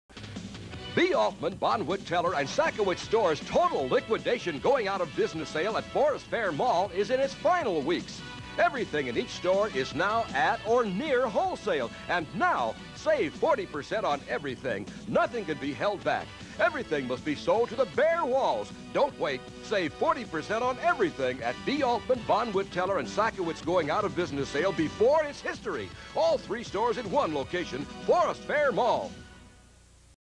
1990 B Altman Bonwit Teller SakoWitz Forest Fair Mall Liquidation Sale Commercial
This was taken from a BetaMax tape of recorded television from 1990.